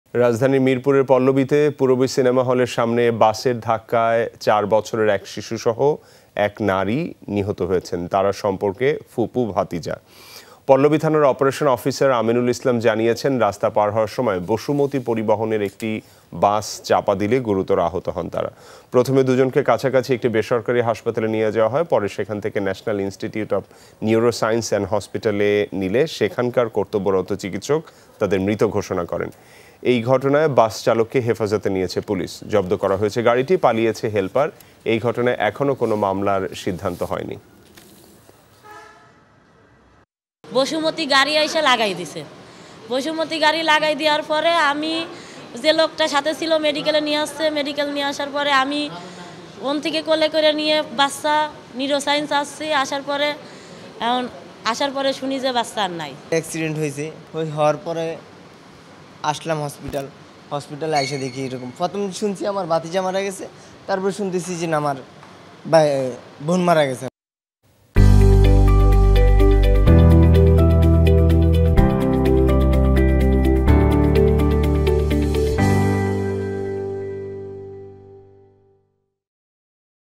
মিরপুরের পল্লবীতে বাসের ধাক্কায় নারী ও শিশু নিহত | Pallabi Accident | Jamuna TV
রাজধানীর মিরপুরের পল্লবীতে, পূরবী সিনেমা হলের সামনে বাসের ধাক্কায় চার বছরের এক শিশুসহ এক নারী নিহত হয়েছেন। তারা সম্পর্কে ফুফু-ভাতিজা। পল্লবী থানার অপারেশন অফিসার আমিনুল ইসলাম জানিয়েছেন, রাস্তা পার হওয়ার সময় বসুমতি পরিবহনের একটি বাস চাপা দিলে গুরুতর আহত হন তারা। প্রথমে দু'জনকে কাছাকাছি এটি বেসরকারি হাসপাতালে নিয়ে যাওয়া হয়। পরে সেখান থেকে ন্যাশনাল ইনস্টিটিউট অব নিউরো সায়েন্সেস অ্যান্ড হসপিটালে নিলে সেখানকার কর্তব্যরত চিকিৎসক তাদের মৃত ঘোষণা করেন। এ ঘটনায় বাস চালককে হেফাজতে নিয়েছে পুলিশ।